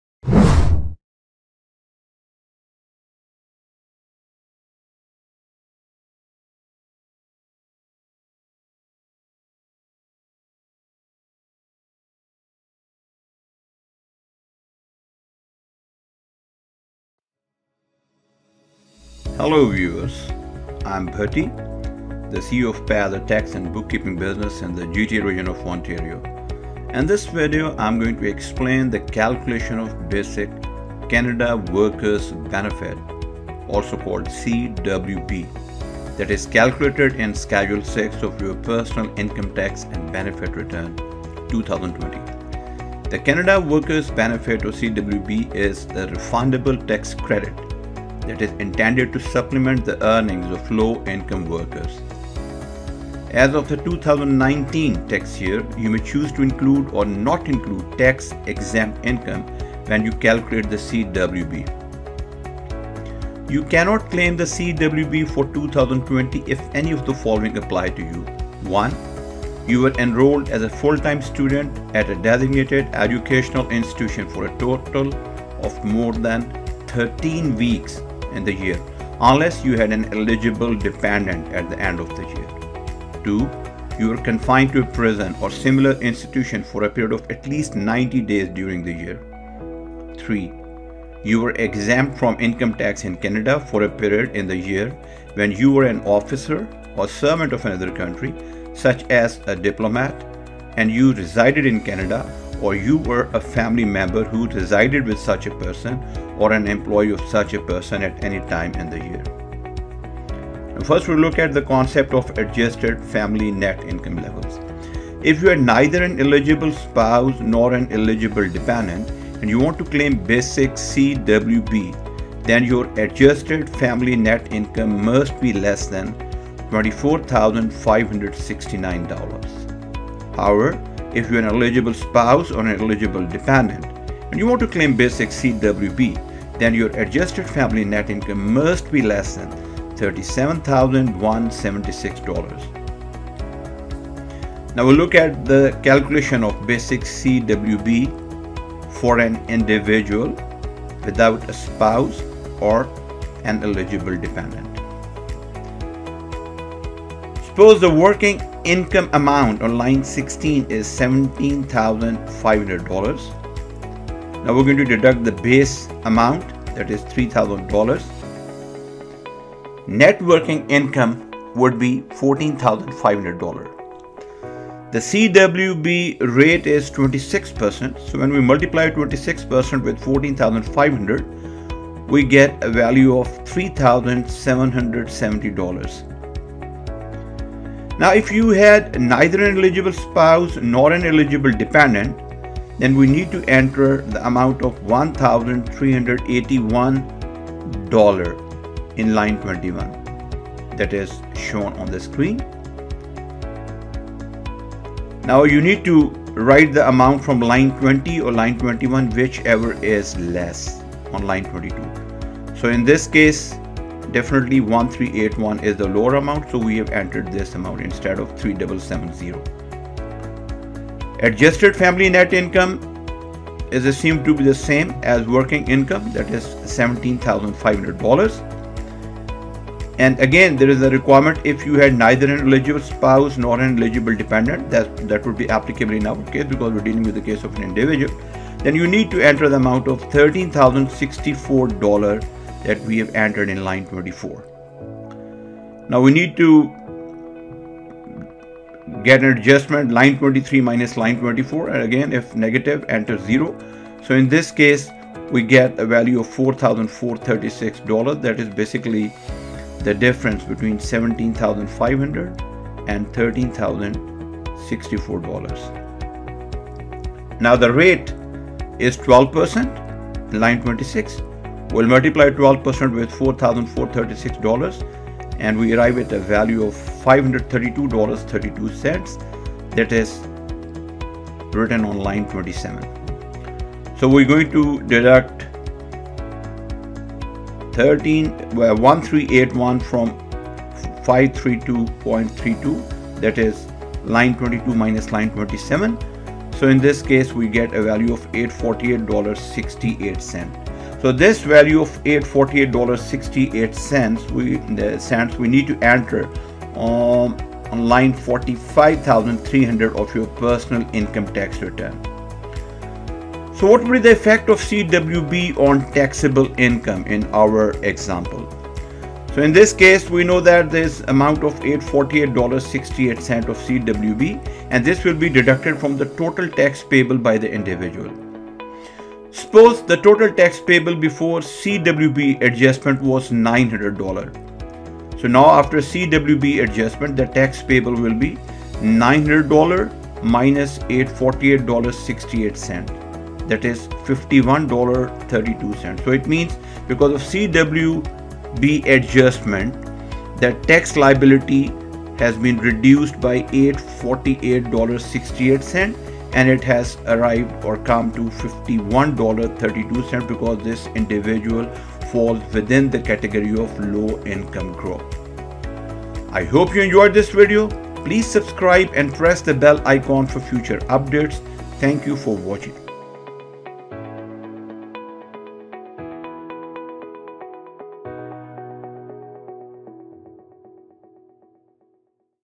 How to calculate basic CWB?
This video briefly explains the calculation of Canada Workers Benefit (CWB) for an individual income tax filer. It explains the concept of CWB and its eligibility criteria.